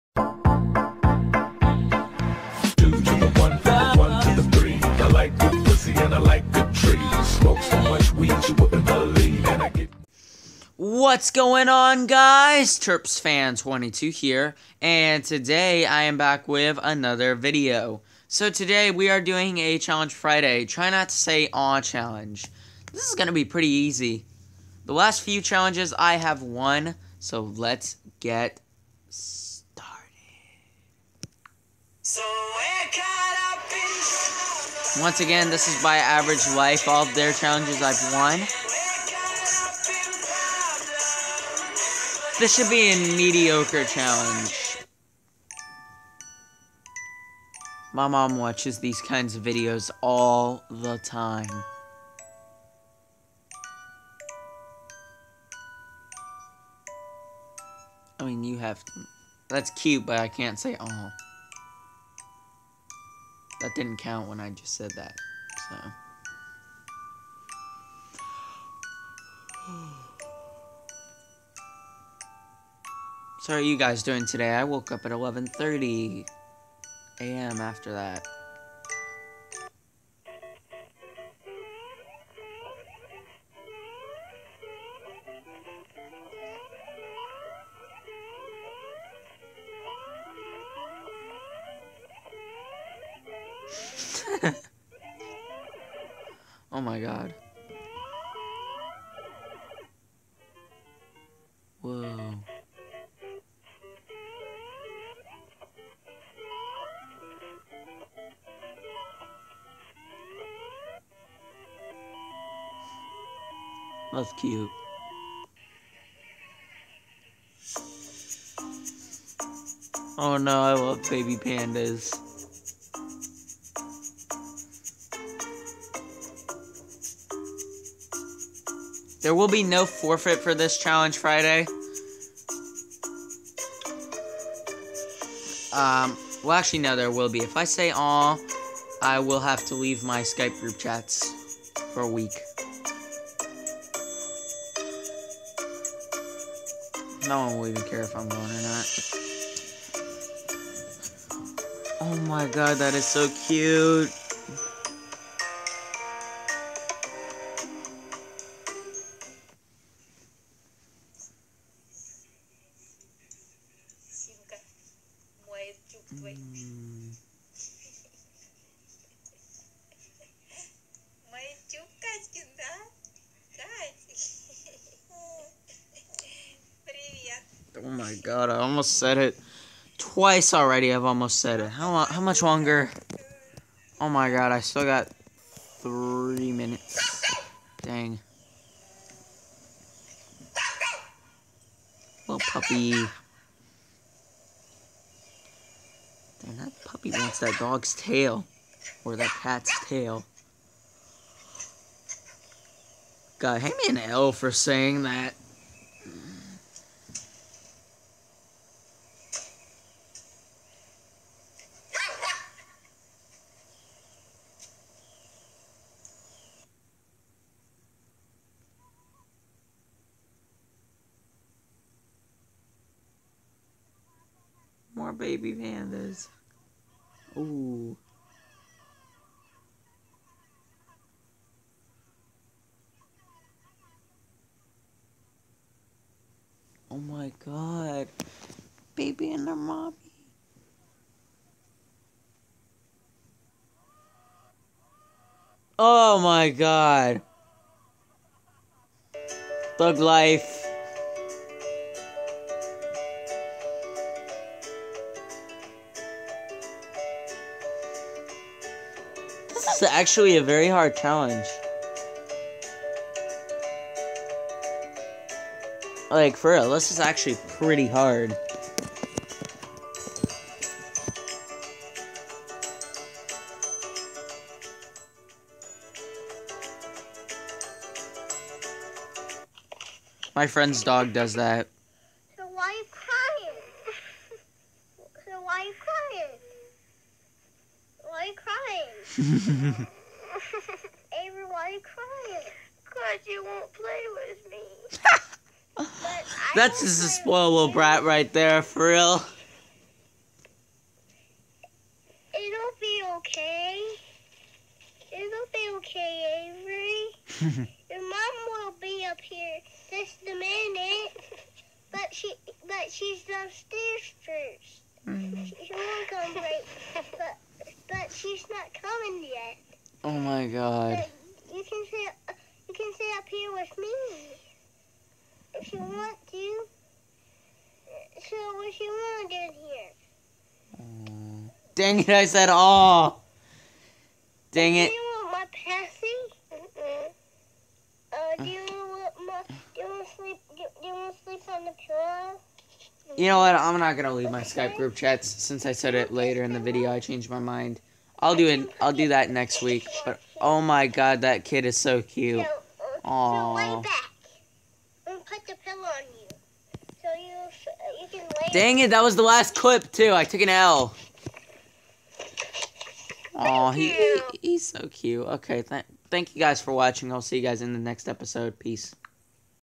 Challenge Friday try not to say aww challenge
Recorded by DU Recorder – Screen recorder for Android I failed I'm not going to do the forfeit in the video yet next week I will do the forfeit if it is an appropriate challenge thank you guys for watching follow me on my Instagram and Twitter personal @mikefobfan101 YouTube @terps_fan22